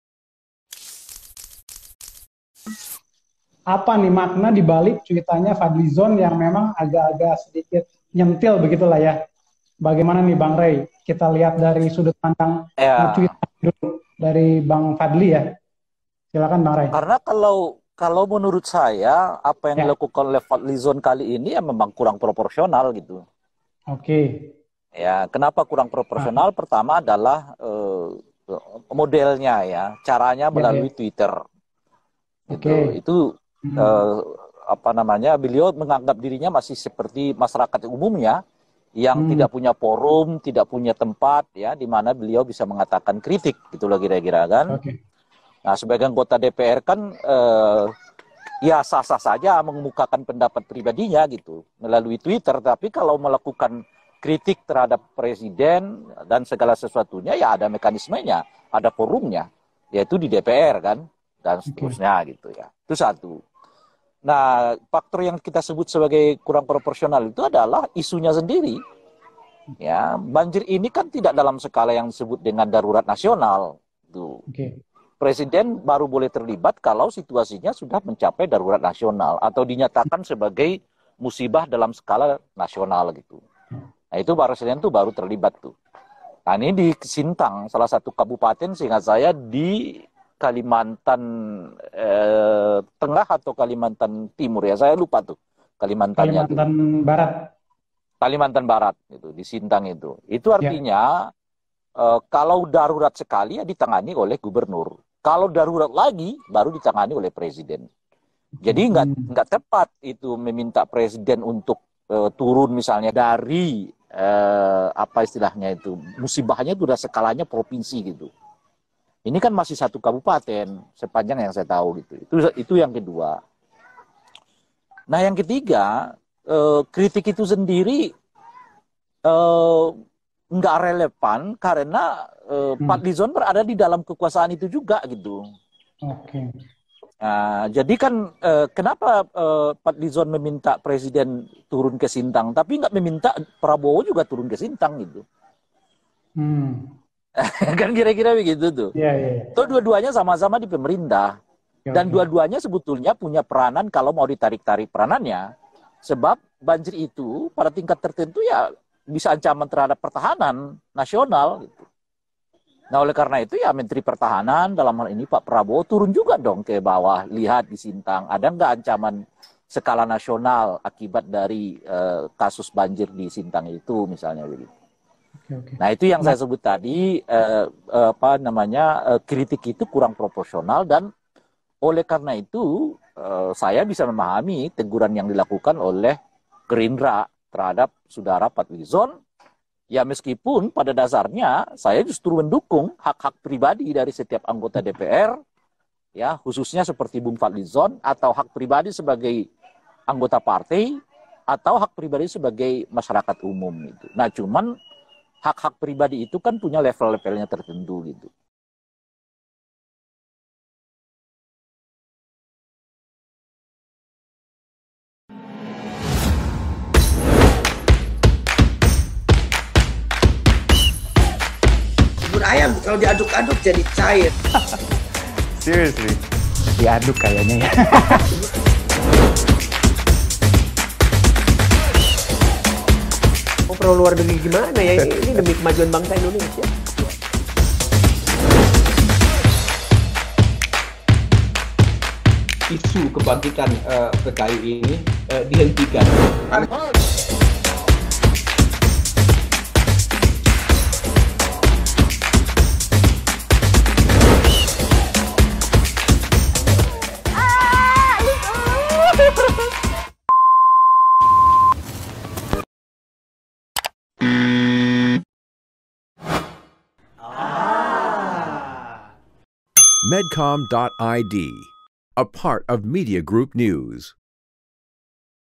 Pengamat: Kritikan yang Dilakukan Fadli Zon Kurang Proporsional
Ketua Umum Partai Gerindra, Prabowo Subianto akhirnya menegur langsung politikus Gerindra Fadli Zon. Hal ini buntut dari cuitan di twitter pada 12 November yang menyindir Presiden Joko Widodo soal banjir di Sintang. Menurut Pengamat Politik Lingkar Madani Indonesia, Ray Rangkuti yang dilakukan oleh Fadli Zon kurang proporsional.

Ada 3 hal mengapa disebut kurang proporsional. Pertama, sebagai anggota DPR cara kritikan yang dilakukan oleh Fadli Zon salah. Kedua, Banjir Sintang tidak dalam skala darurat nasional. Ketiga, kritikan tidak relevan.